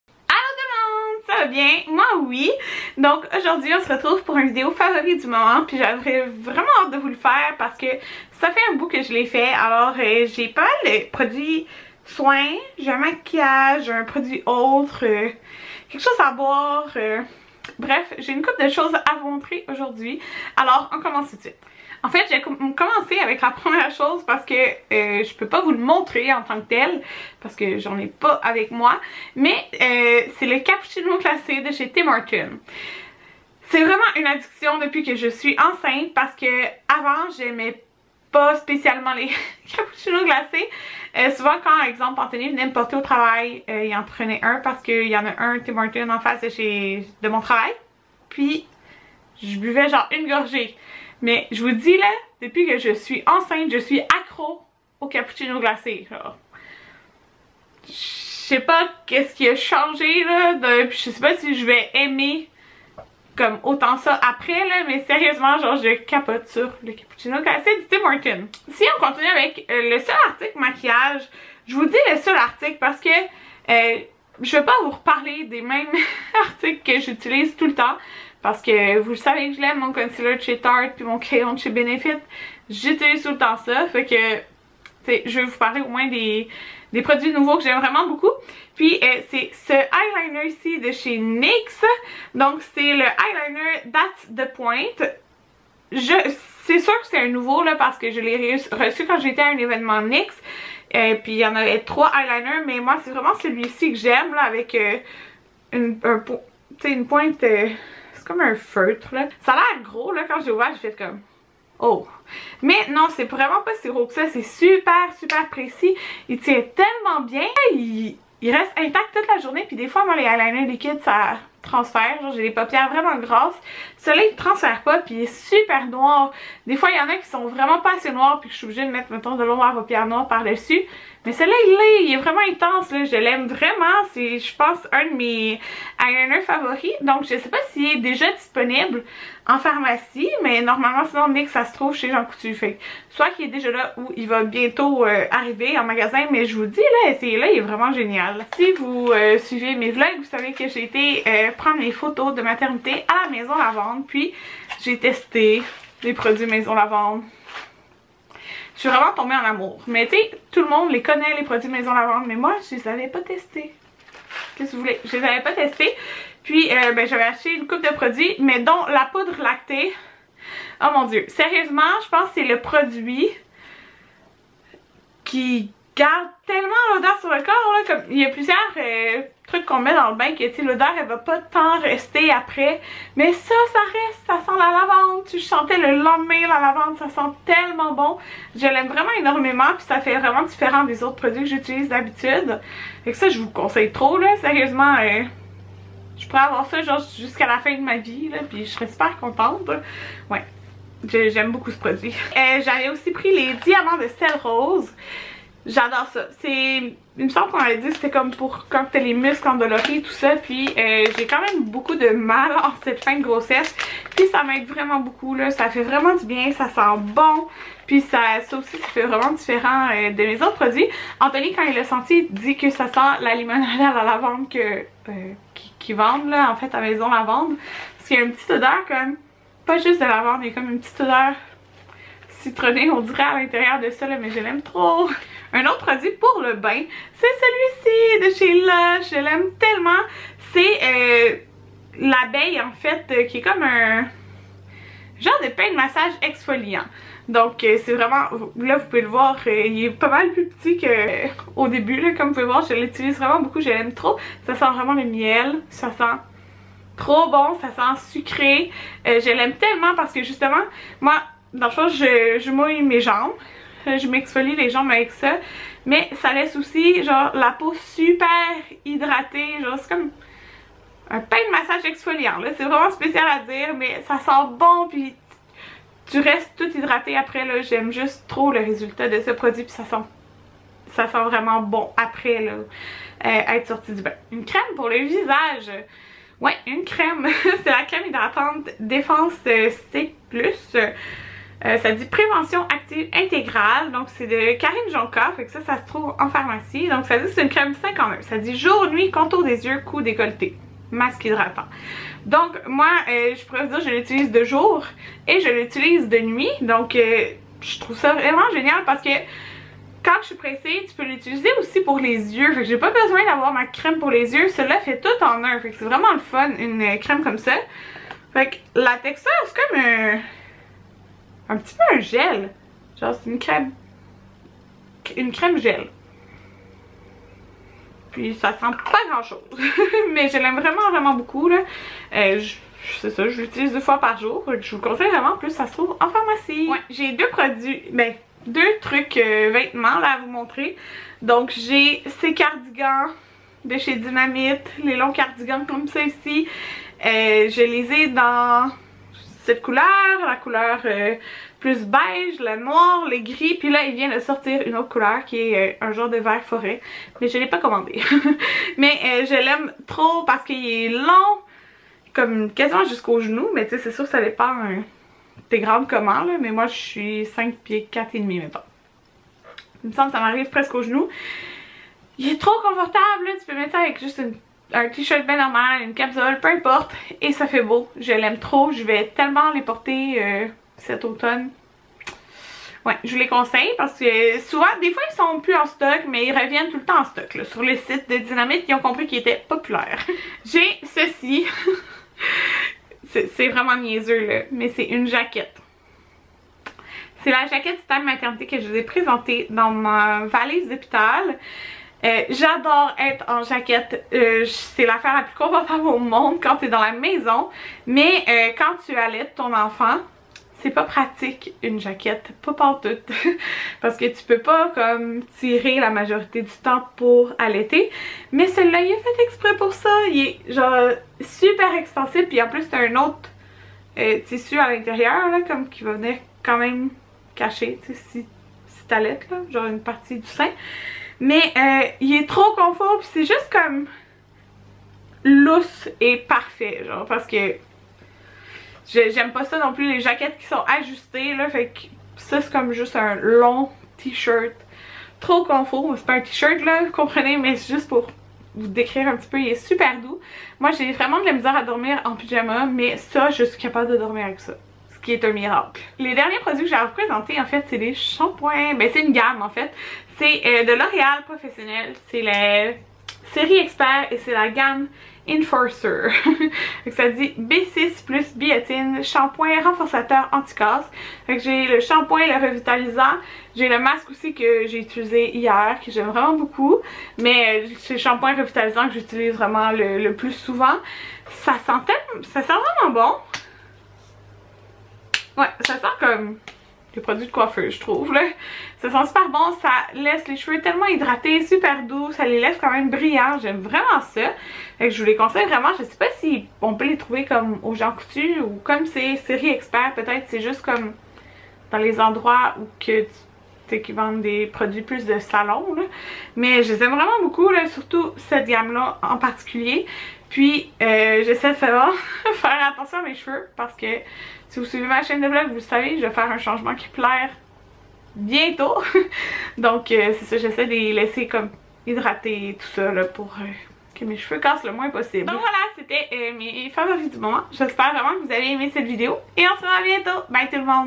Favoris du moment // Maquillage, Soins et +
Allo tout le monde ! Aujourd'hui je vous présente mes favoris du moments côté Maquillage, Soins, Vêtements et + .

M E S   L I E N S:
♡ Chaîne de vlogs:

A U T R E S
♡ Caméra avec laquelle je tourne:
Canon G7X